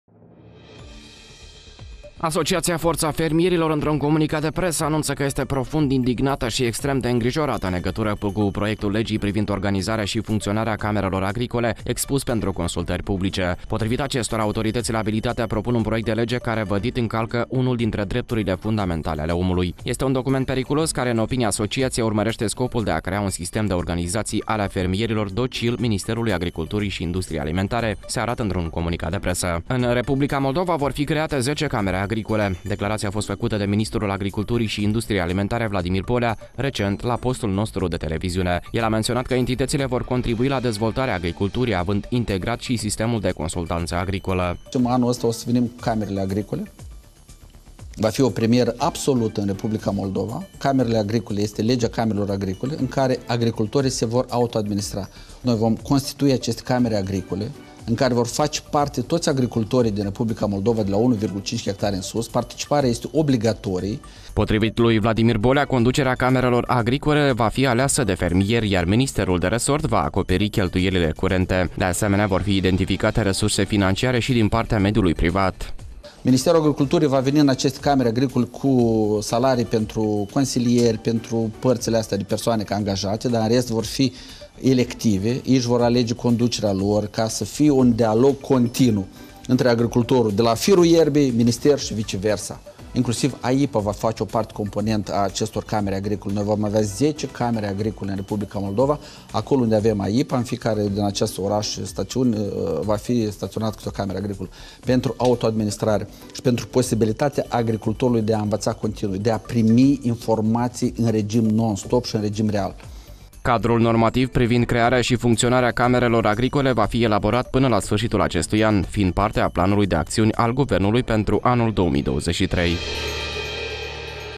Fermierii, nemulțumiți de „Camerele agricole” - AGRO TV News
Asociația Forța Fermierilor, într-un comunicat de presă, anunță că este profund indignată și extrem de îngrijorată în legătură cu proiectul legii privind organizarea și funcționarea camerelor agricole, expus pentru consultări publice.